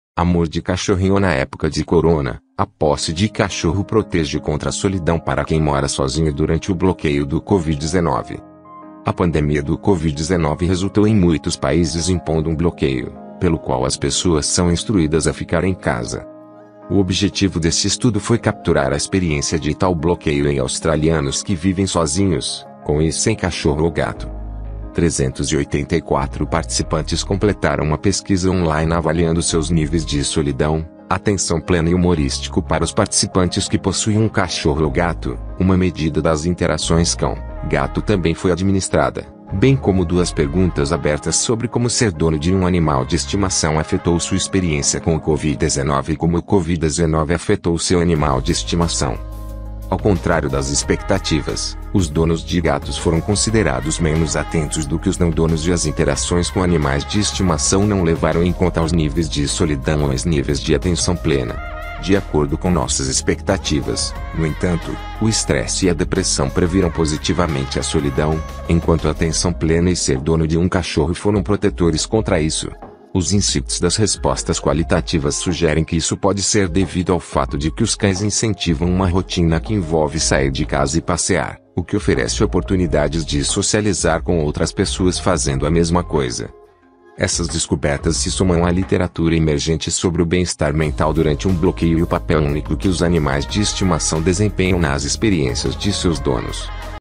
A posse de cachorro protege contra a solidão para quem mora sozinho durante o bloqueio do covid 19.
A posse de cachorro protegeu contra a solidão para quem mora sozinho durante o bloqueio do covid 19.
Amor de cachorrinho na época de Corona: a posse de cachorro protegeu contra a solidão para quem mora sozinho durante o bloqueio do COVID-19.
A pandemia do COVID-19 resultou em muitos países impondo um 'bloqueio', pelo qual as pessoas são instruídas a ficar em casa.
O objetivo deste estudo foi capturar a experiência de tal bloqueio em australianos que vivem sozinhos, com e sem cachorro ou gato.
Trezentos e oitenta e quatro participantes completaram uma pesquisa online avaliando seus níveis de solidão, atenção plena e humor. Para os participantes que possuíam um cachorro ou gato, uma medida das interações cão/gato também foi administrada, bem como duas perguntas abertas sobre como ser dono de um animal de estimação afetou sua experiência com o COVID-19 e como o COVID-19 afetou seu animal de estimação.
Ao contrário das expectativas, os donos de gatos foram considerados menos atentos do que os não donos e as interações com animais de estimação não levaram em conta os níveis de solidão ou os níveis de atenção plena. De acordo com nossas expectativas, no entanto, o estresse e a depressão previram positivamente a solidão, enquanto a atenção plena e ser dono de um cachorro foram protetores contra isso. Os insights das respostas qualitativas sugerem que isso pode ser devido ao fato de que os cães incentivam uma rotina que envolve sair de casa e passear, o que oferece oportunidades de socializar com outras pessoas fazendo a mesma coisa.
Essas descobertas se somam à literatura emergente sobre o bem-estar mental durante um bloqueio e o papel único que os animais de estimação desempenham nas experiências de seus donos.